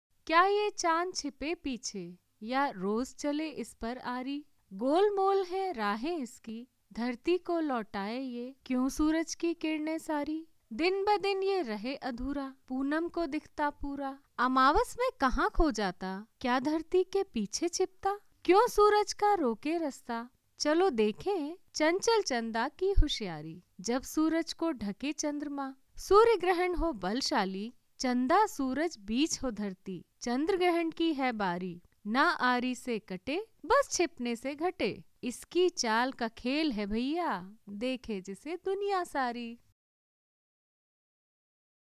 POEM ON MOON by Akram sheikh studio
A lunar eclipse occurs when the Moon passes directly behind the Earth into its umbra (shadow). This can occur only when the sun, Earth, and moon are aligned (in "syzygy") exactly, or very closely so, with the Earth in the middle. Hence, a lunar eclipse can occur only the night of a full moon. The type and length of an eclipse depend upon the Moon's location relative to its orbital nodes.
by Akram sheikh studio